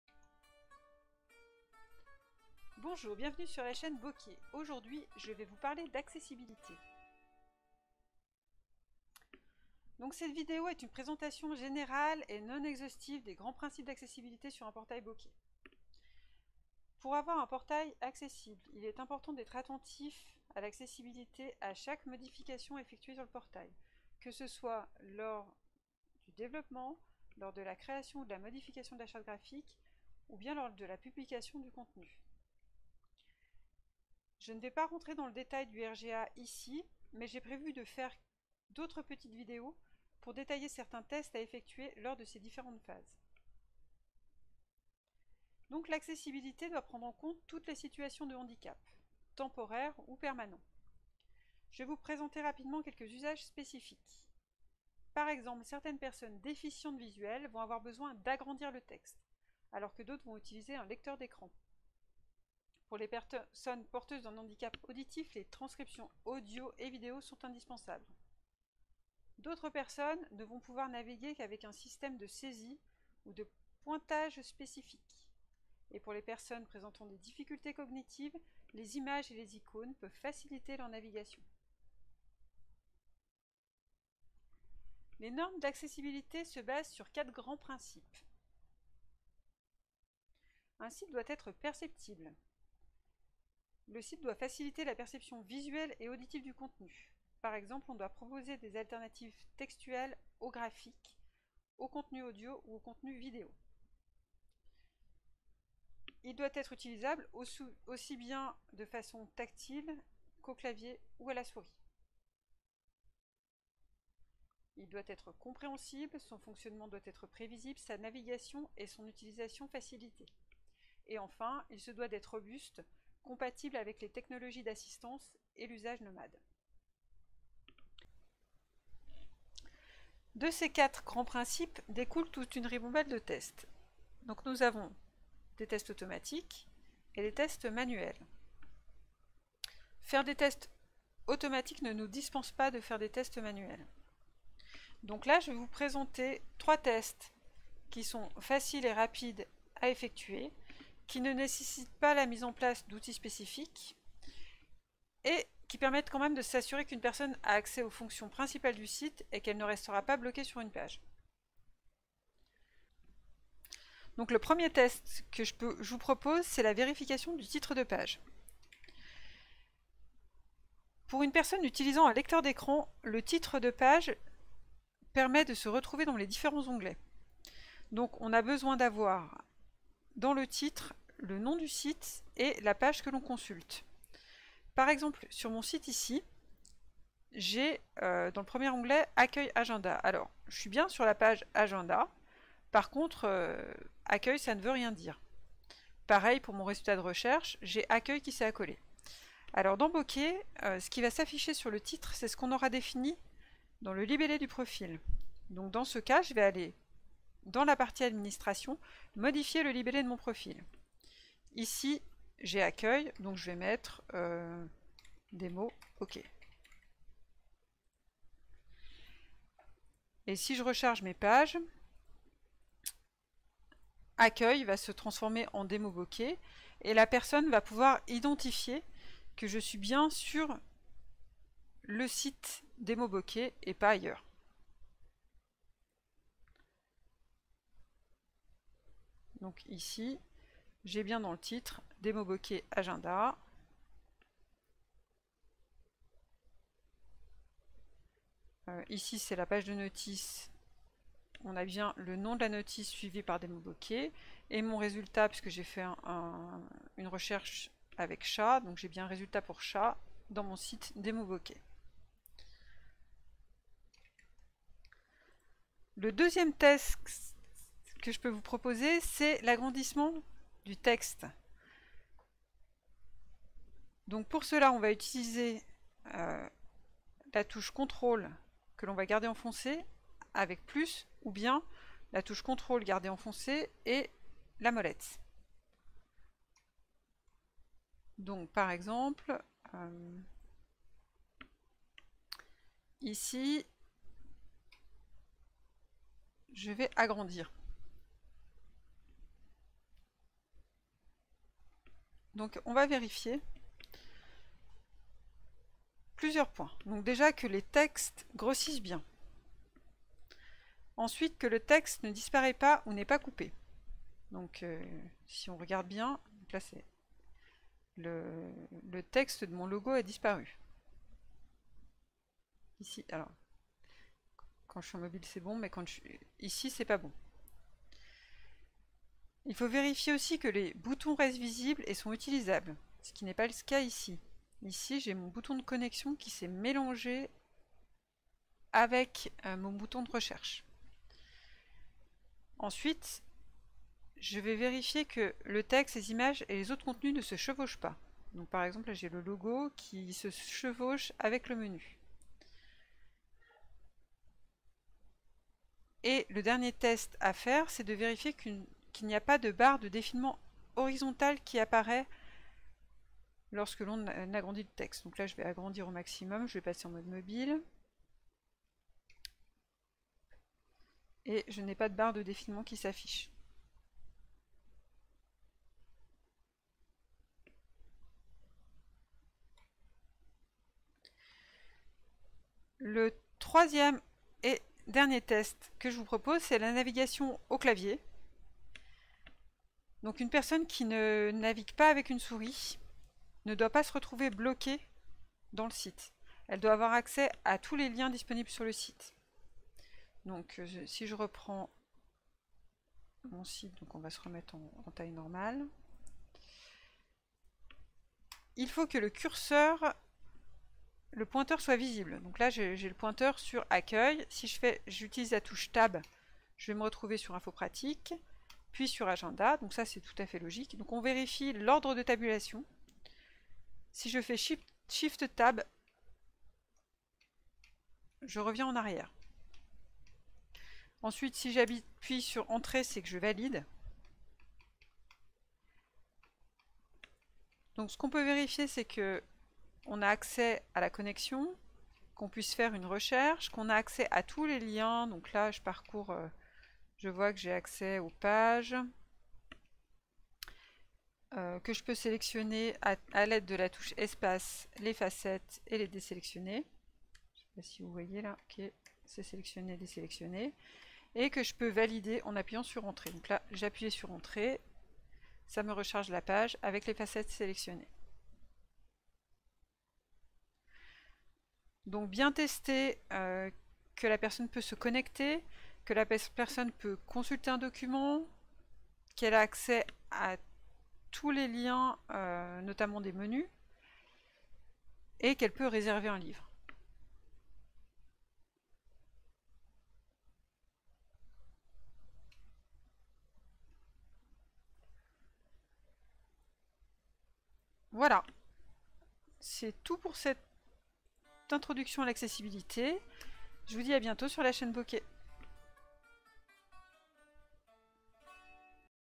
Bokeh : Accessiblité, introduction
Cette vidéo présente les bases de l'accessibilité dans un site bokeh.
0:00 Introduction
0:47 Différents profils a prendre en compte
1:25 Les 4 grands principes de l'accessibilité
2:18 Tests manuels
2:47 Test : Le titre de page est-il cohérent ?
4:29 Test : Peux t-on agrandir le texte sans nuire a la navigation ?
6:32 Test : Peux t-on utiliser le site avec le clavier  uniquement ?

Les sous titres en français sont disponibles.